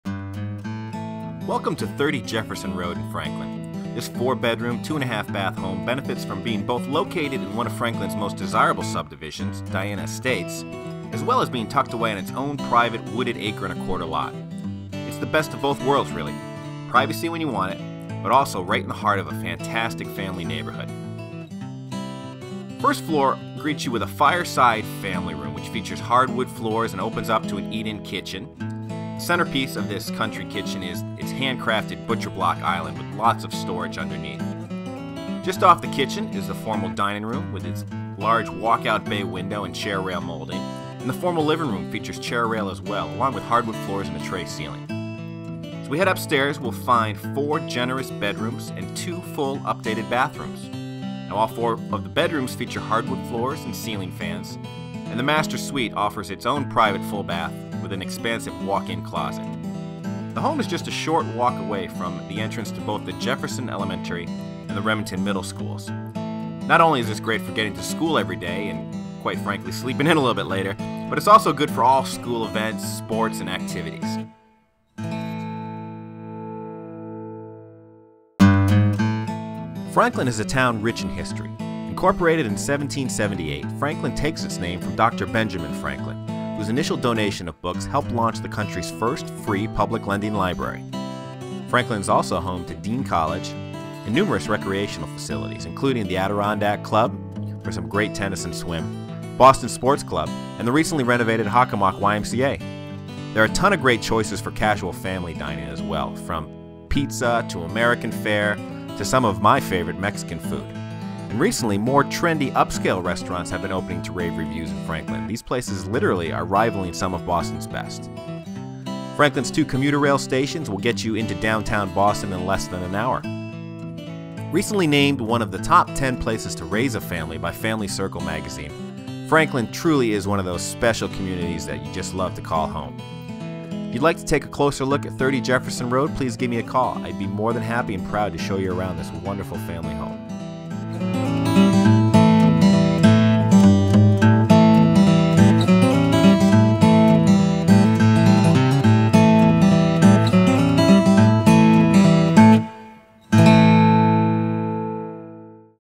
30Jefferson.com
A video tour of 30 Jefferson Road, Franklin, MA and the community of Franklin, MA